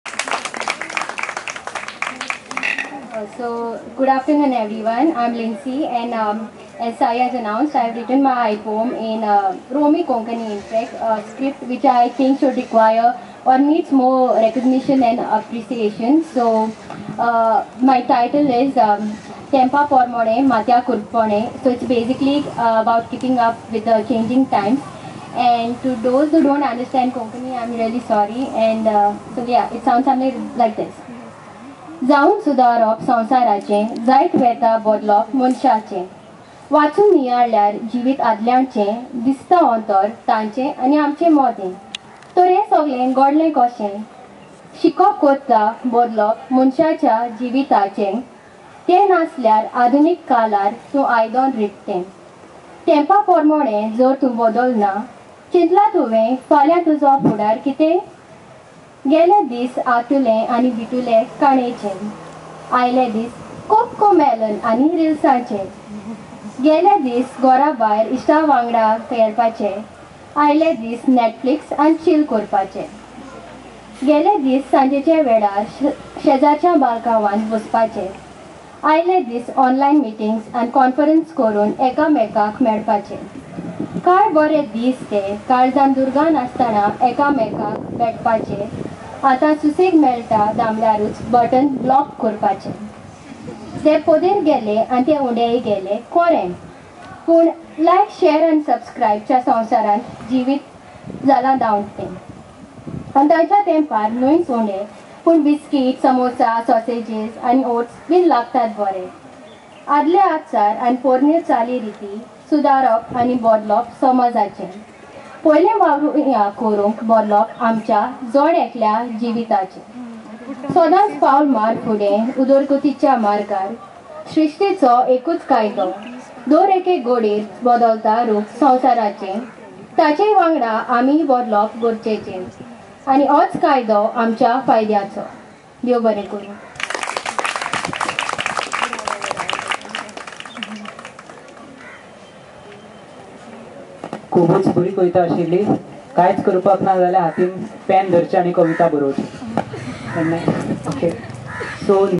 Konkani poem -- Tempa porm'monem. At GoaU Pure Poetry event, Oct 12, 2023.
This festival was started in 2017 by Aaron S Pereira and his class... It brings poetry buffs from the varsity (across disciplines) together. This year it was held on October 12, 2023, Thursday evening, starting just after 2 pm. Recorded by Frederick Noronha (FN).